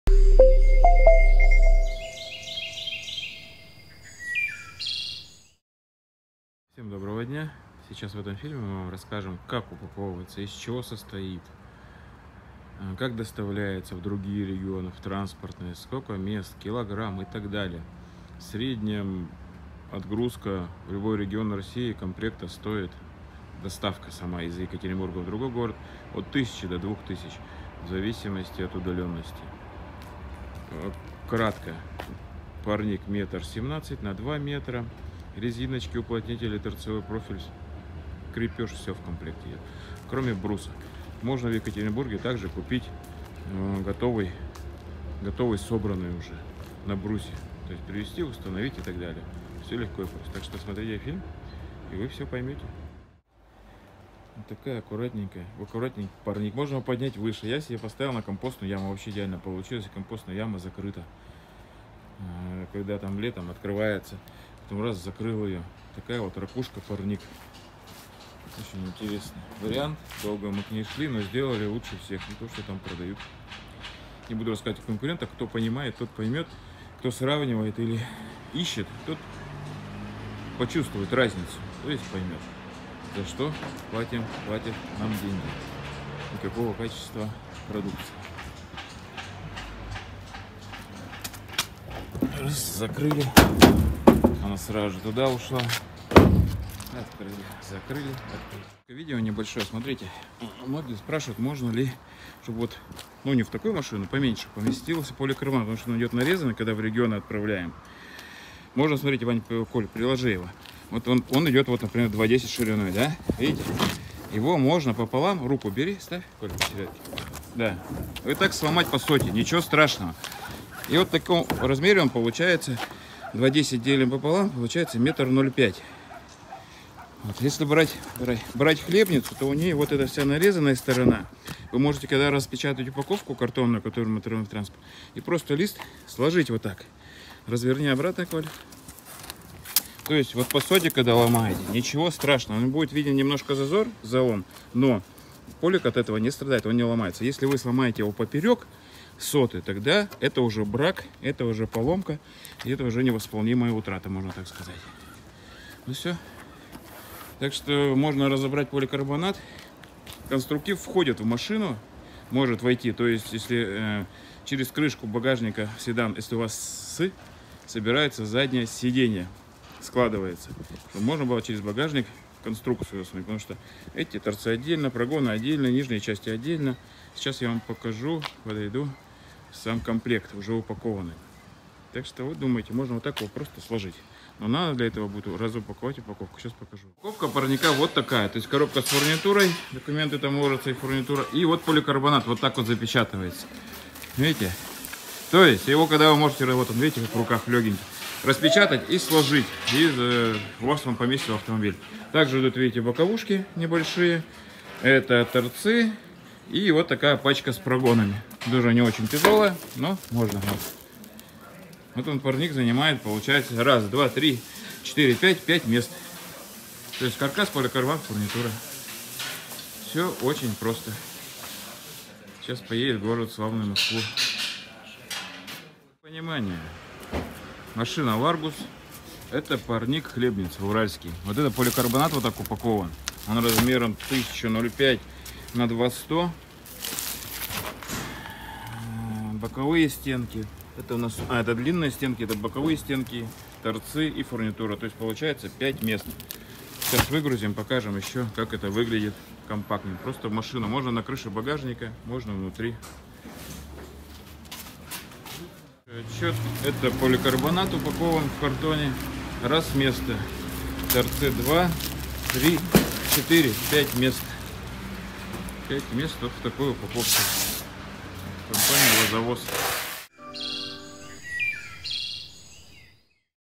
ПАРНИК ХЛЕБНИЦА. УПАКОВКА И ДОСТАВКА В РЕГИОНЫ.
Хлебница упаковка, доставка

Все спрашивают: Доставка? Упаковка? Сколько стоит?
Отвечаем!
1️⃣. Упаковка в стретч пленку, пупырчатую и картон стоит 1000₽
2️⃣. Доставка карьер склад - склад транспортной компании 1000₽
3️⃣. Доставка из Екатеринбурга до Вашего города от 1000₽ до 2000₽ в зависимости от региона и города

А теперь о самом парнике

❤️ Парник хлебница «Уральский»
❤️ Самый главный плюс парника, его обслуживание, использование всей площади парника и компактность.
✅ Размеры: ширина 1170 мм, длина 2000мм. Высота 800мм.
✅ Каркас изготовлен из оцинкованной профильной трубы 20х20х1,2мм и полимерно порошковой окраски, цвет 8017 шоколад
✅ Стоимость :
1️⃣ Каркас с фурнитурой = 9700 рублей.
2️⃣ Комплект: каркас  с спк Актуаль Био 4мм = 12500 рублей .
3️⃣ собранный  парник на брусе 100/100 = 15810 рублей

Если вы из другого города и вам нужна доставка транспортной компанией, то прибавится:
Упаковка 1000 рублей.
Доставка до тк по Екатеринбургу 1000 рублей. Доставка до вас примерно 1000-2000  рублей.

Если вы решили приобрести парник, контакты ниже👍

🏡 Екатеринбург ул Климовская 23
🖥 ural-usadba.ru